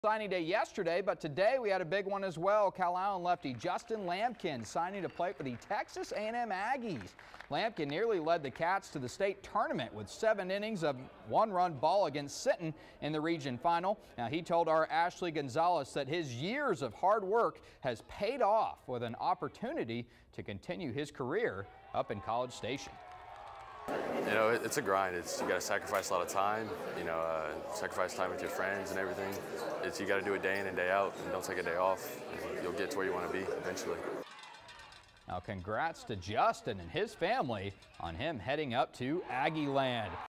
Calallen's Lamkin signs with Texas A&M - 3Sports
The Calallen ace lefty signed with the Aggies as he looks to lead the 'Cats to the state tournament this season.